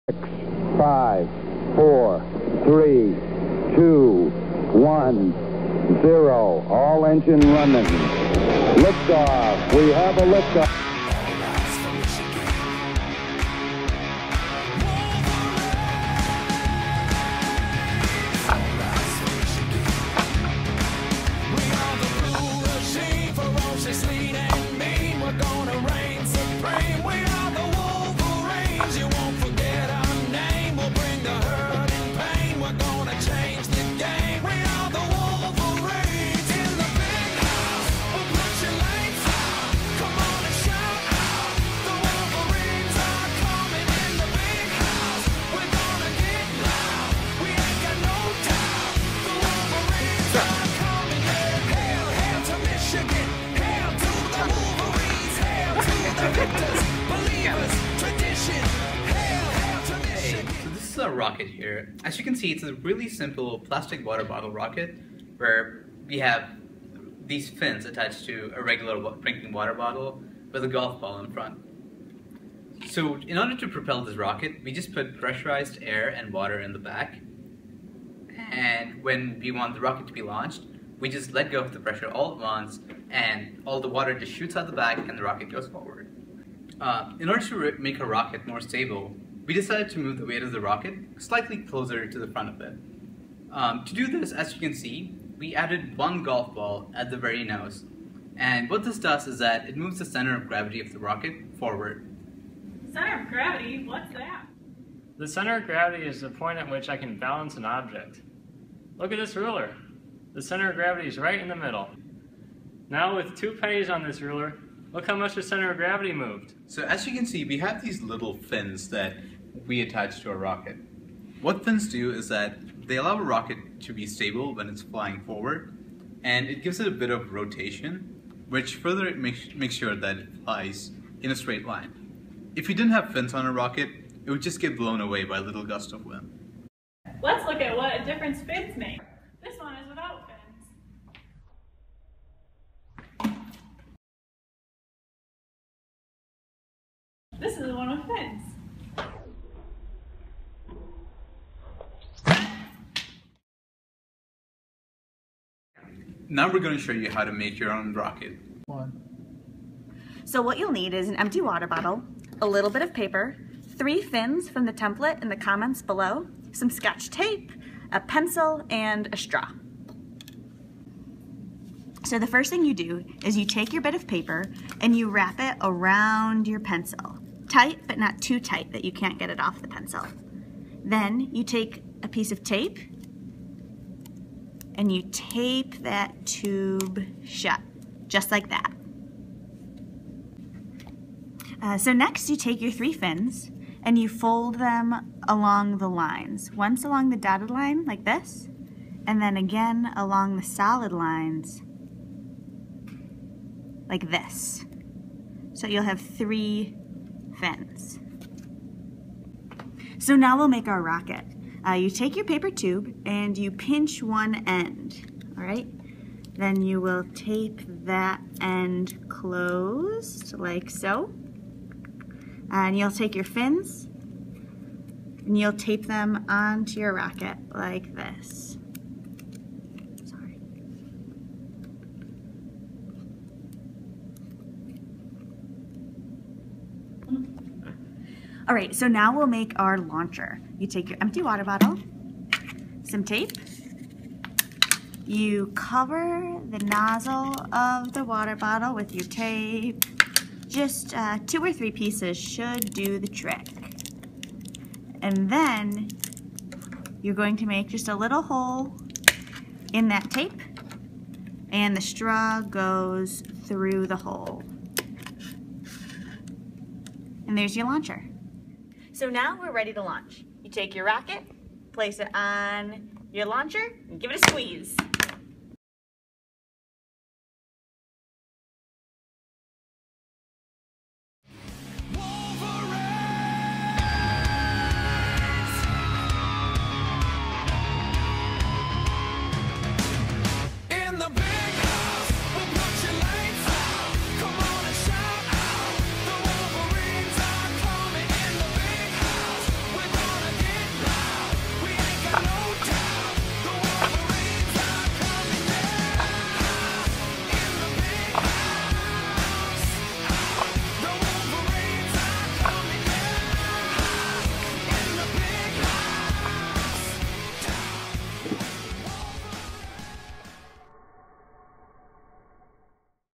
Bottle Rocket Science for Middle Schoolers
ME 495 Lab 4 Outreach Video for Team 1, Section 8

Watch to learn about rocket science, learn how to make your own rocket, and see cool slow-motion water bottle rocket launches. Target audience: 6th grade students.